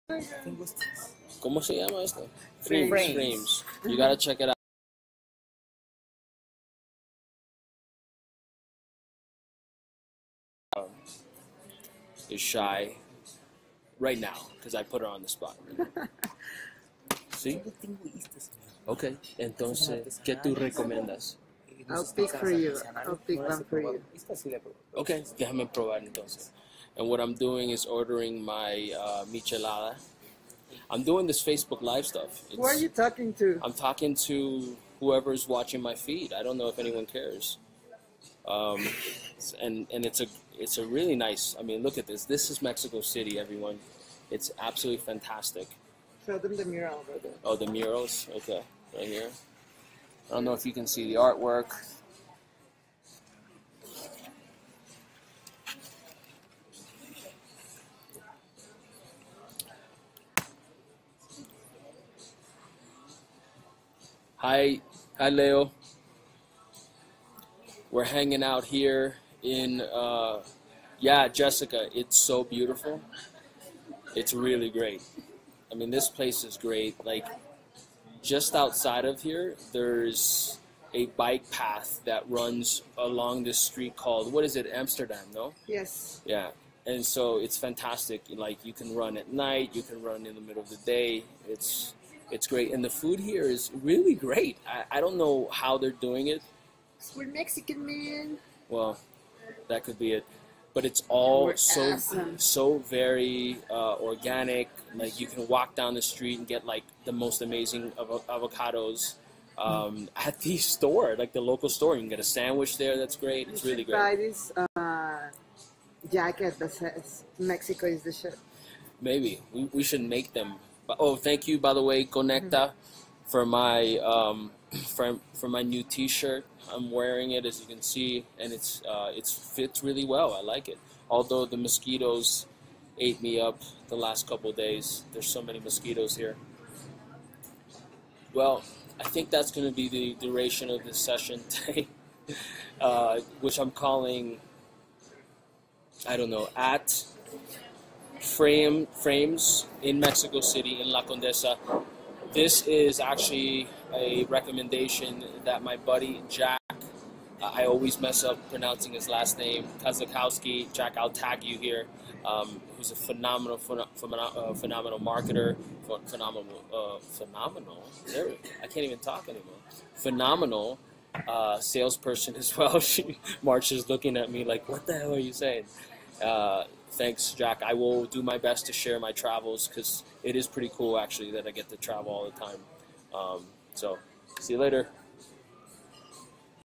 This place is a fantastic lunch spot that offer sick Micheladas"
in the Mexico City neighborhood of "La Condesa" - Thanks for my new shirt Rich Peniche of Conekta